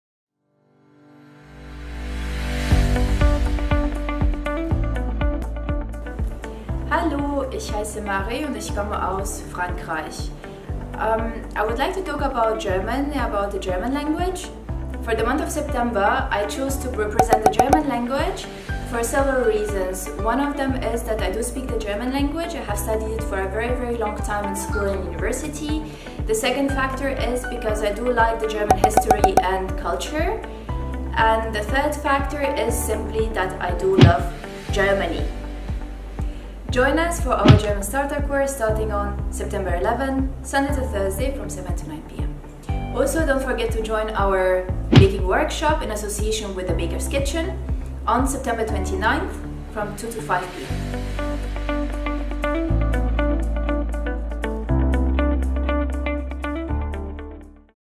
My Language Challenge: Why German?
We are back with another LanguageChallenge. Here is Marie giving us a list of why she loves the German Language. Yes, 2 weeks! 📖📒😊